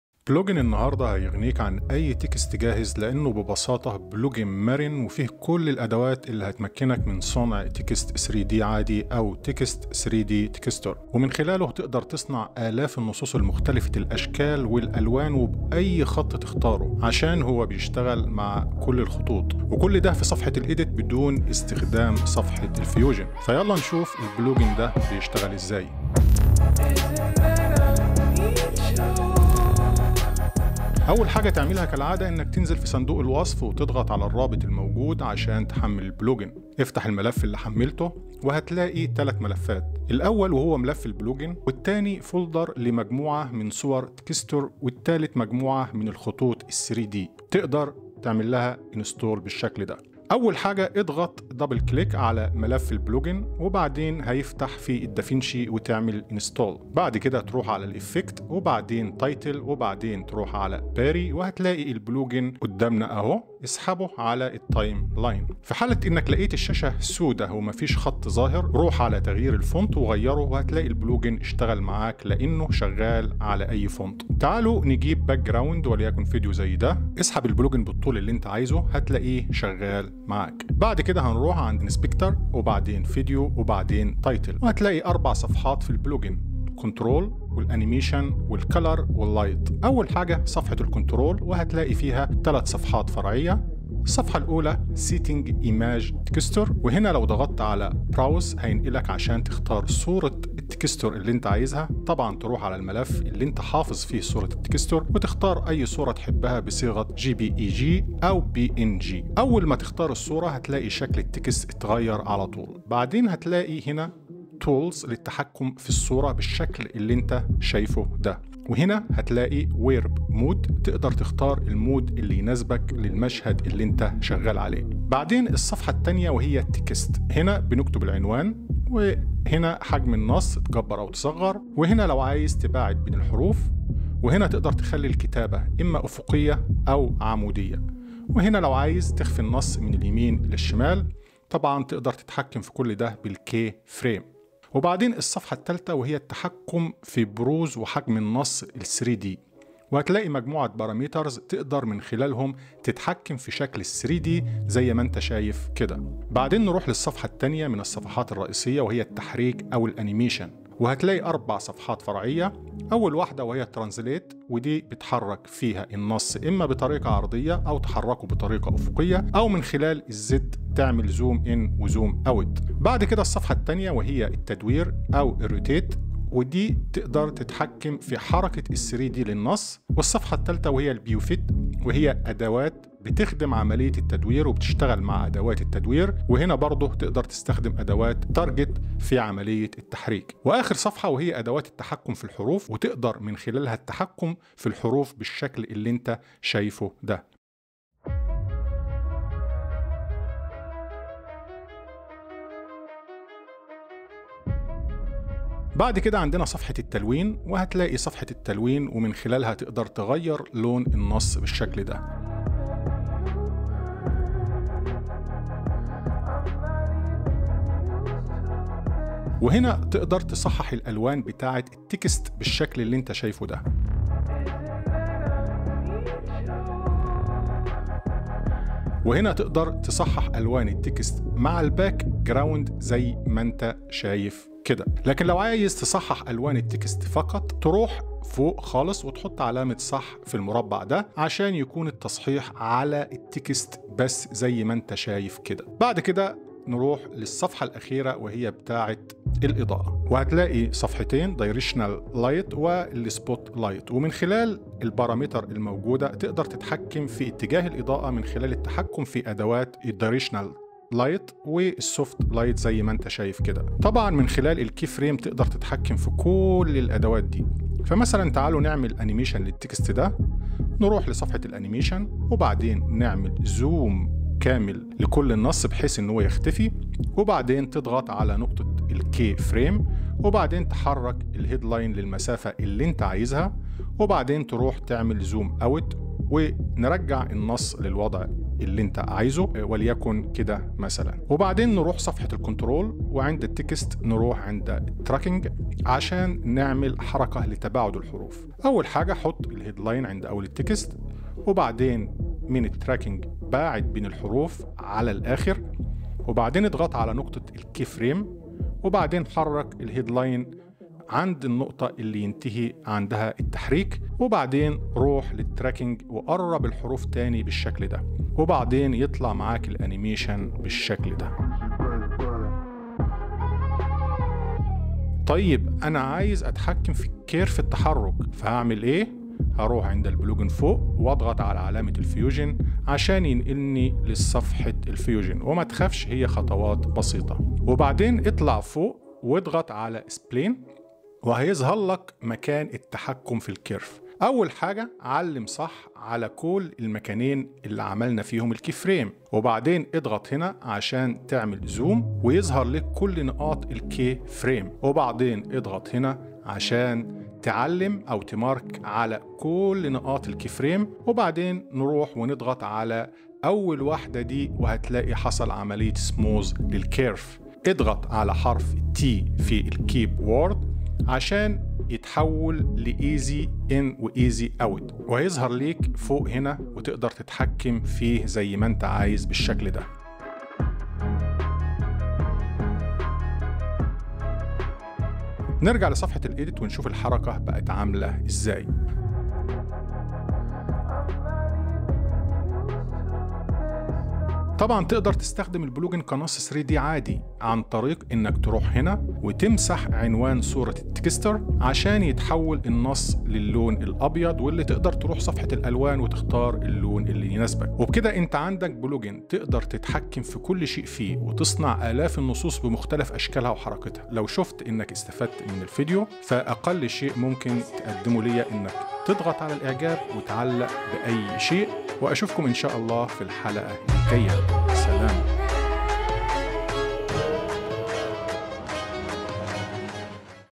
3D Text Texture - PLUGIN in EDIT Page - Davinci Resolve 18.6 - دافنشي ريزولف 18.6
رابط تحميل البلوجن ..
رابط تحميل البلوجن للداعمين..
الان يمكنك عمل الالاف من  عناوين 3D بكل أنوعها  في صفحة التحرير في برنامج دافنشي ريزولف . إنه بلوجن مرن ومنظم وسهل الاستخدام يمكنك من خلاله خلق اي عنوان ثلاثي الابعاد مع أي صورة بصيغة jpeg أو png . كما يمكنك استخدمة في خلق عناوين ثلاثية الابعاد عادية واختيار اللون المناسب . كما يمكنك التحكم في التحريك ثلاثي الابعاد بكل سهولة وايضا يمكنك التحكم في الاضائة .

اتمنى ان يكوم هذا المنتج مفيد لكم.